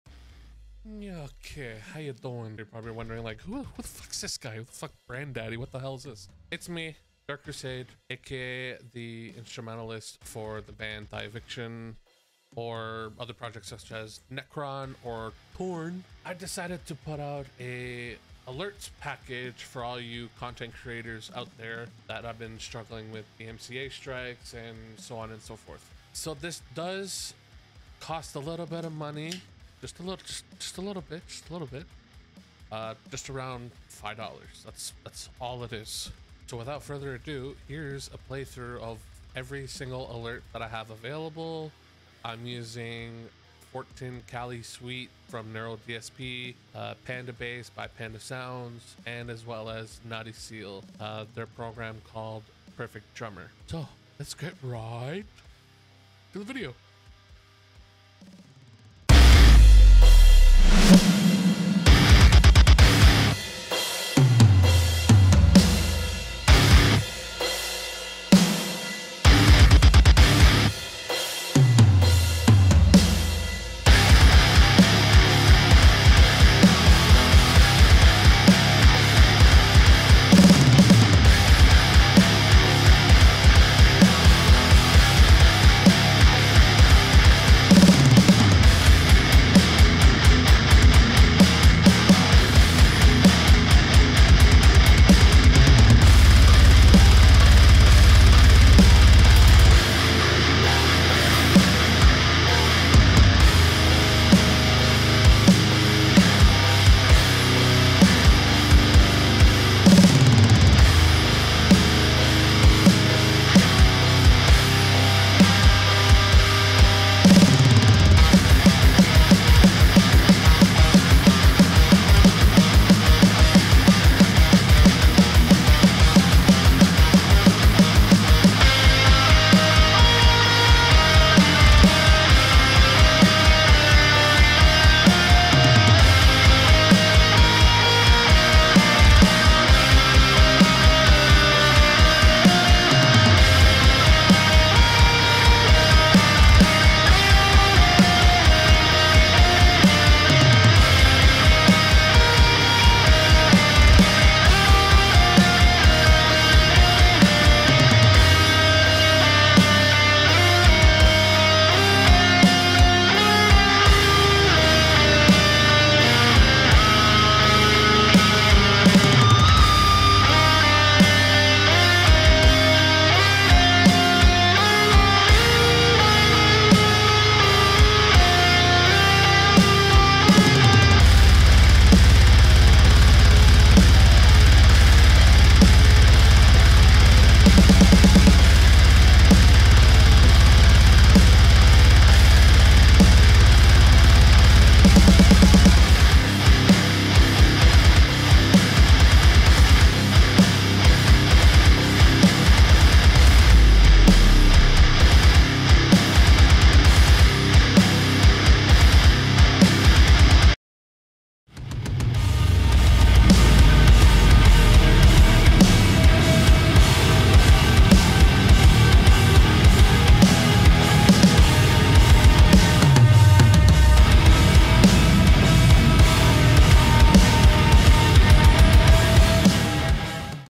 Content Creator Alert Sounds Package!
For all content creators whether you're on YouTube, Twitch, Kick etc etc.
BEHOLD... My first ever Alerts Sound Package for your streams, or recording your next upcoming YouTube Video!

List of Gear Used:
Guitar: Jackson JS32 8 String Dinky

Alert 1: 0:59
Alert 2: 1:14
Alert 3: 1:28
Alert 4: 1:43
Alert 5: 1:58
Alert 6: 2:08
Alert 7: 2:23
Alert 8: 2:38
Alert 9: 3:01
Alert 10: 3:15